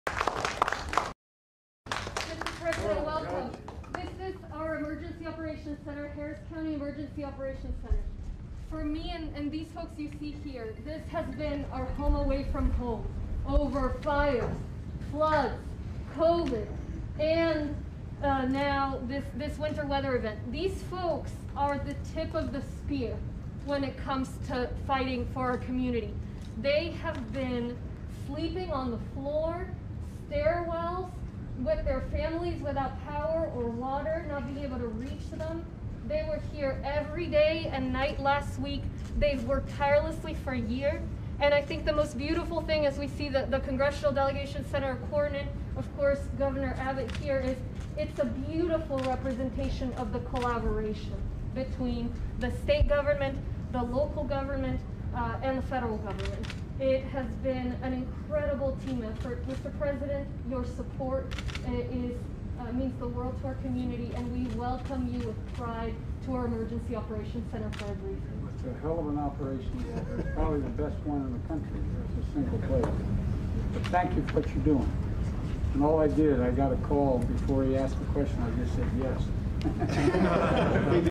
Presient Biden calls Harris County OEC 'probably the best in the country'
During his visit to the Harris County Emergncy Operations Center, President Biden told Harris CountyJudge Lina Hidalgo that the faciliy, “It’s a hell of an operation you have here, probably the best in the country”